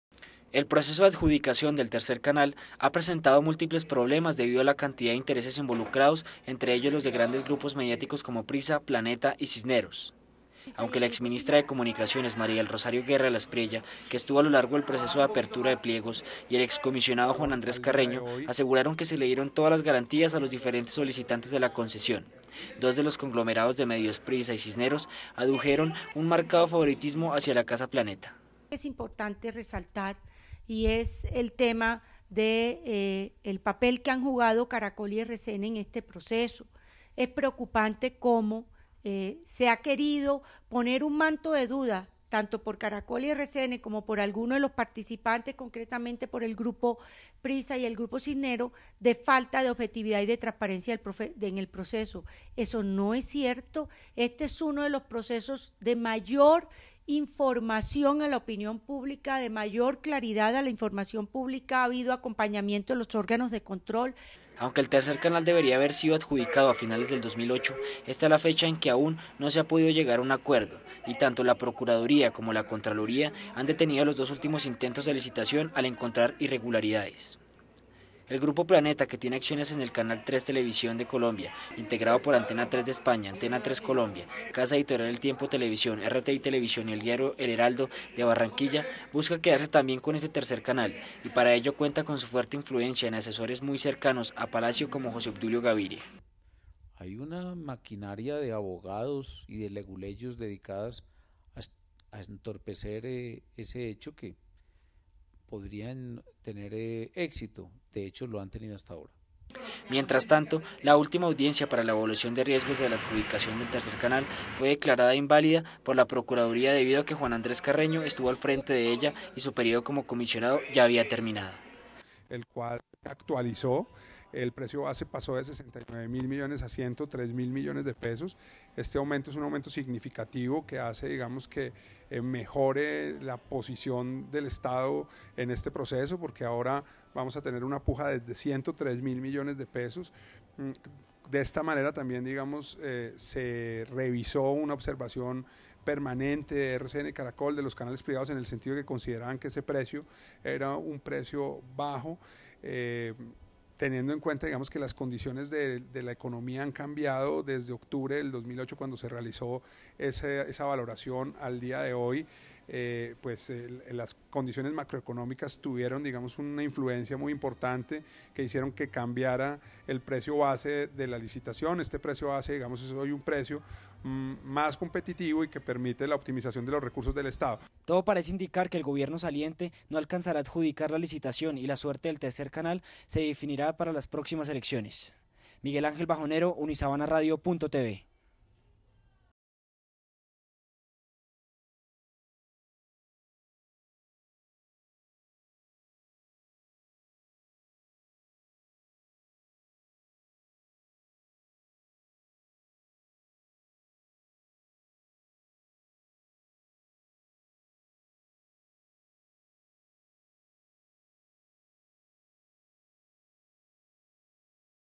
Avanza con lentitud adjudicación del Tercer canal
Innumerables inconvenientes se han presentado en el proceso de adjudicación del tercer canal en Colombia.